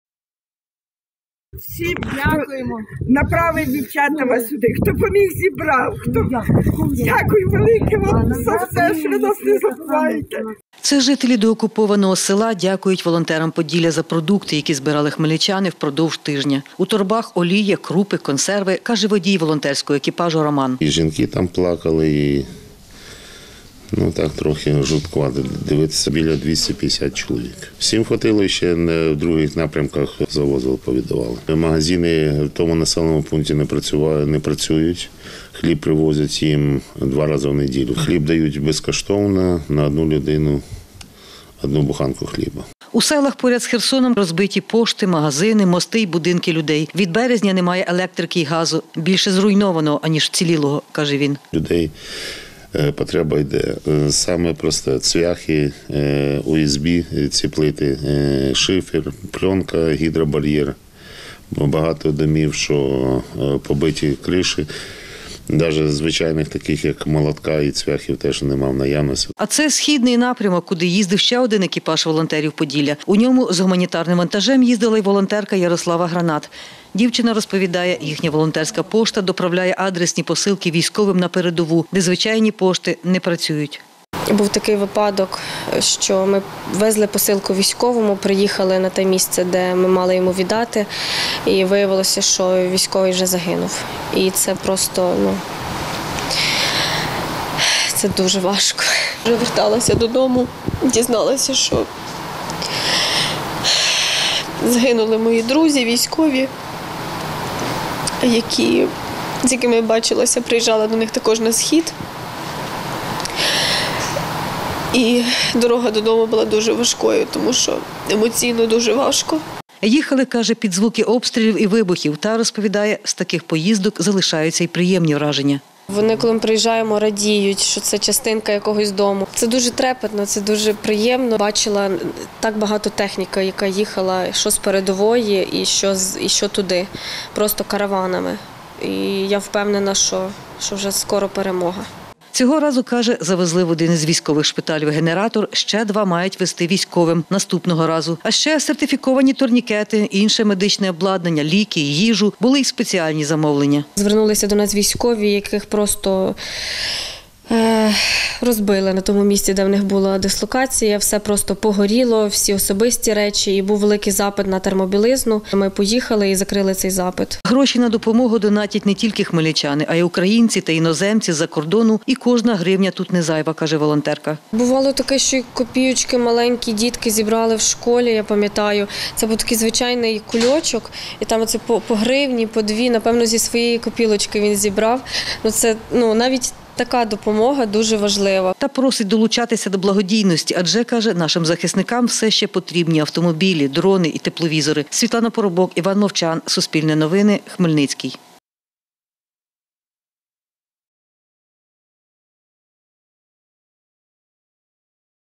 Волонтери їздили до військових на схід та до цивільних в деокуповані населені пункти
Волонтери Поділля повернулися з крайньої поїздки в гарячі точки України, куди їздили з гуманітарним вантажем розповідає волонтерка Ярослава Гранат. За її словами, цього разу їздили два екіпажі до військових на схід та до цивільних в деокуповані населені пункти на південь.